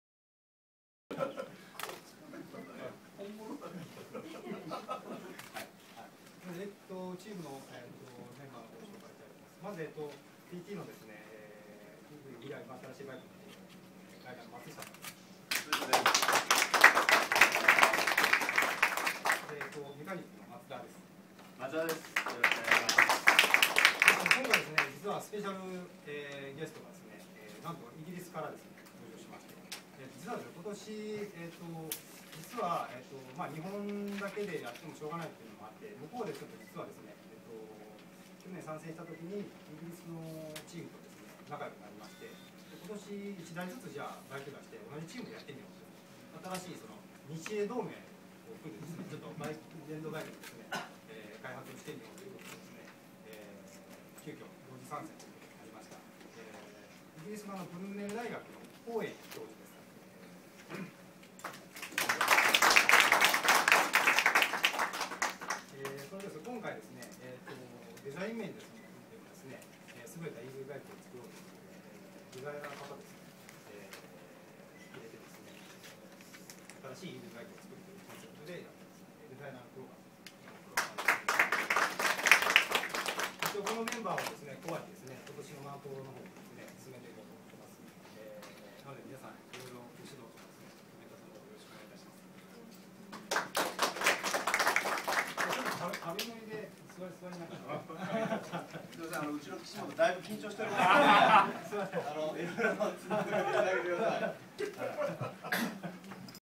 ダダ漏れ アキバの電気街からEVバイクのマン島TT参戦電撃発表 その1
アキバモーターショーCafé トークライブ
「アキバの電気街からEVバイクのマン島TT参戦電撃発表」
その1

マン島ＴＴゼロプロジェクトリーダー　岸本　ヨシヒロ　氏
ライダー／モーターサイクルジャーナリスト　松下　ヨシナリ　氏
インダストリアルデザイナー　黒川　祐介　氏